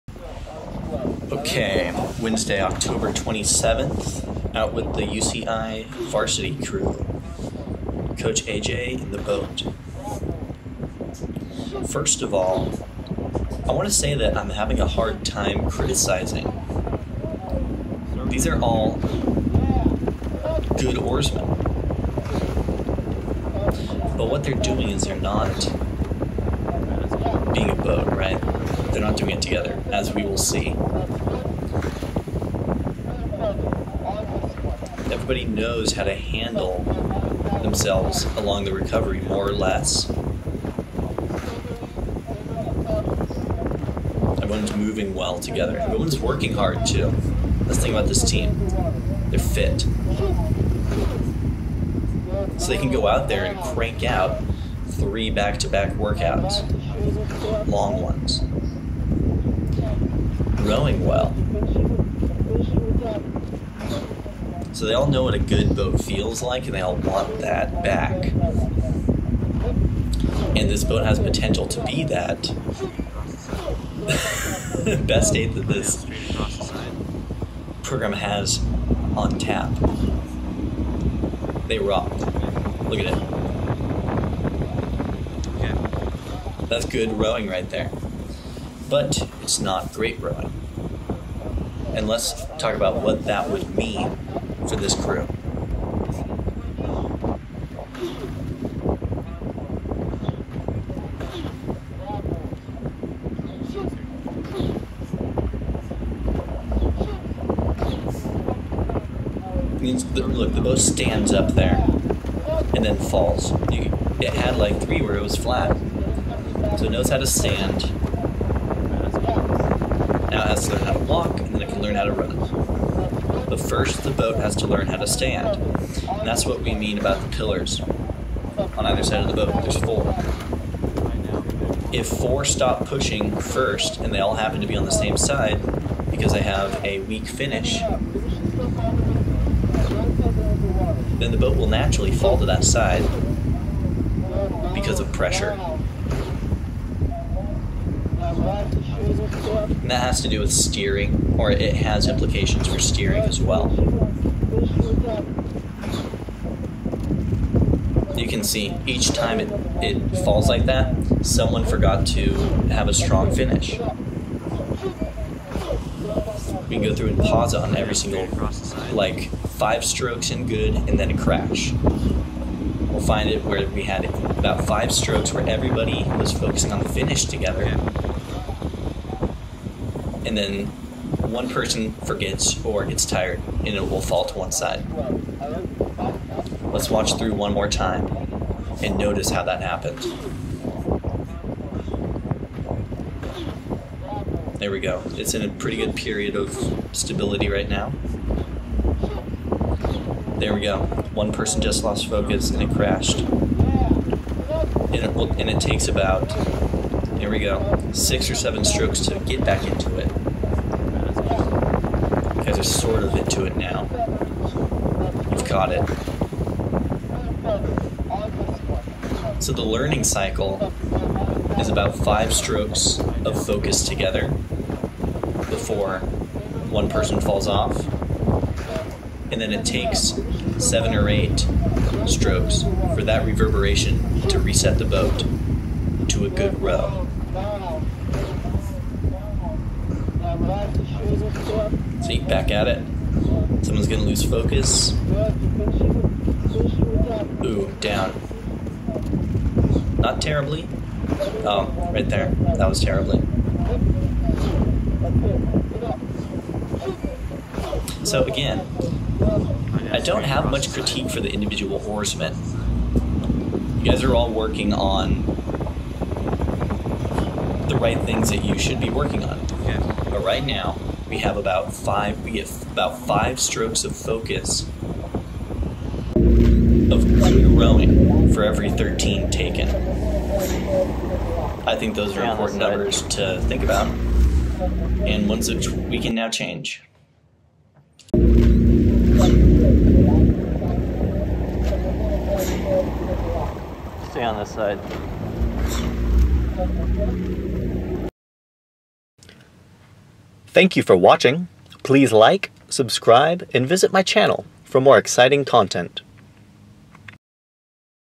Commentary 10/27/21 - UCI Rowing
Some thoughts on the row this morning. This footage was taken while coaching a rowing session with UC Irvine Rowing varsity men ft Coach AJ Brooks filling in seven seat. It is great working with this crew.